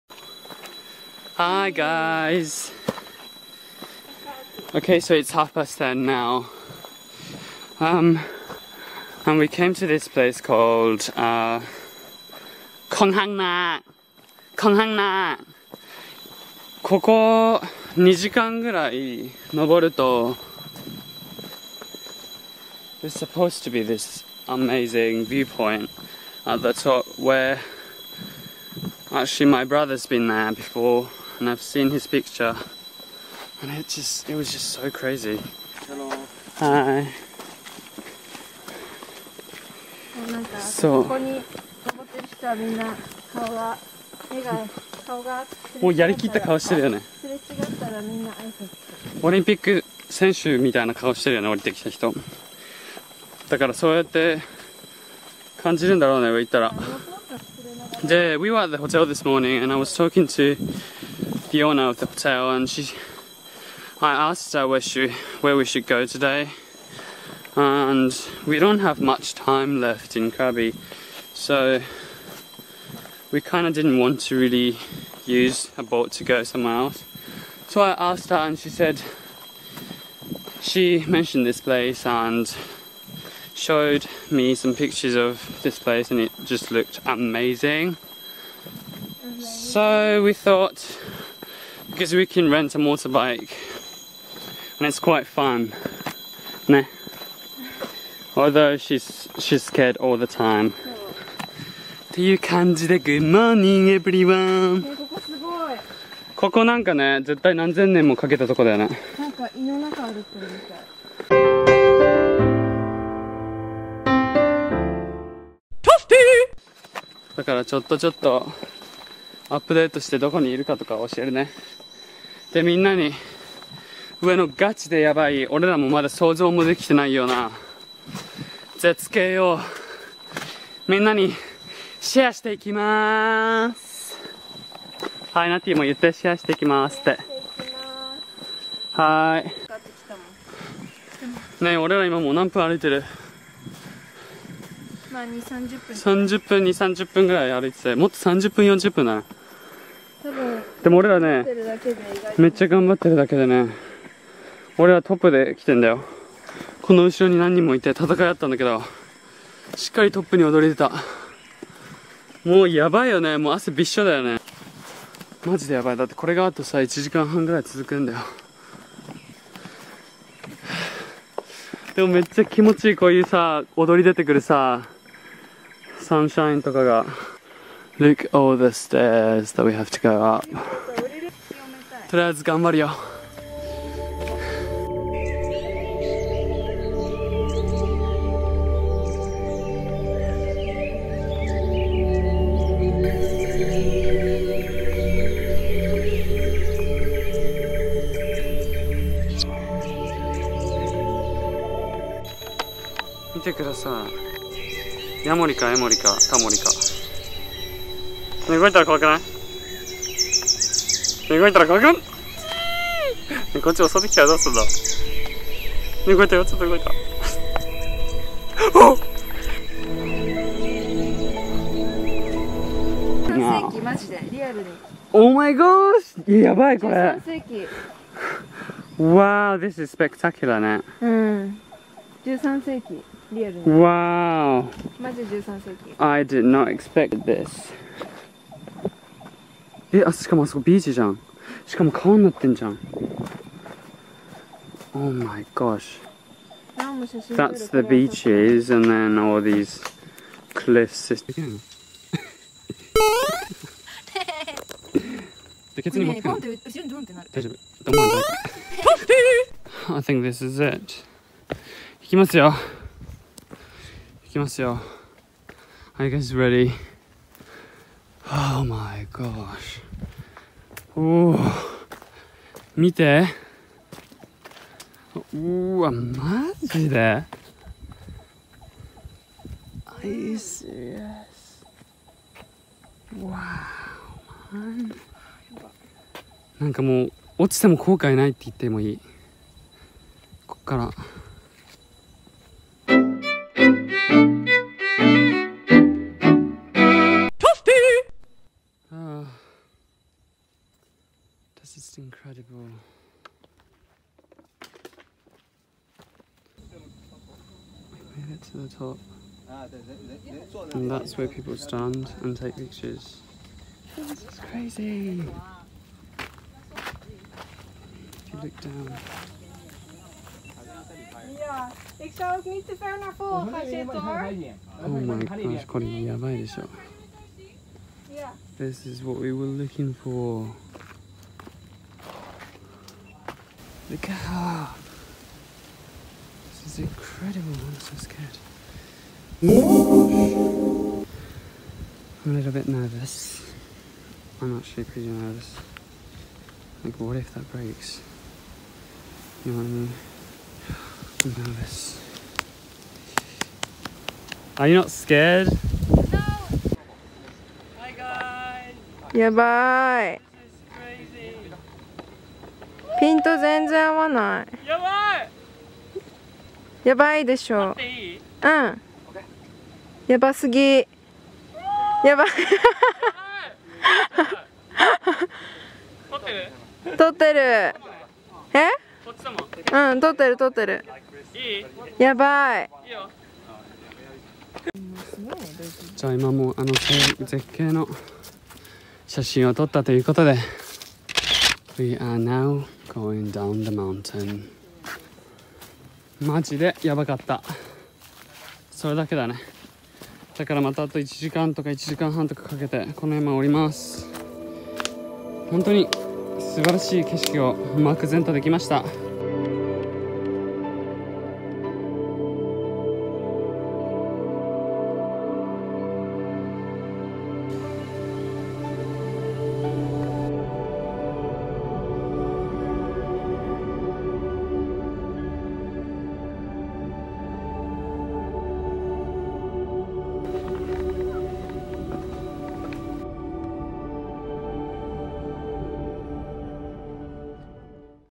【超絶景!!】 命がけでタイの絶景スポットへ
ここはまだ観光客に知られてなくて、ネットとかで探しても情報が少なくて、冷静に秘境なんだけど、とてもやばい。まず登山だけで2時間半くらいかかるし、道のり険しいレベル８９だからビーサンとかできちゃったりされても困るのでしっかり途中下山お願いしてますこんばんは。でも本当にその登山の大変さをまかなってくれるのがそのスポットからの超絶景。こういうタイプの絶景は今まででも見たことないからすごく大興奮だったしすごい感動して息を飲んだ。それなりの気合は絶対必要だからね。タイ南部にあるクラビ県にある、Dragon crest で検索してみれば少しは情報出てくるかもね。行ってみてね！命だけは大切にしてほしいが。まだまだタイをみんなで楽しもう！さんはい、最高〜〜！！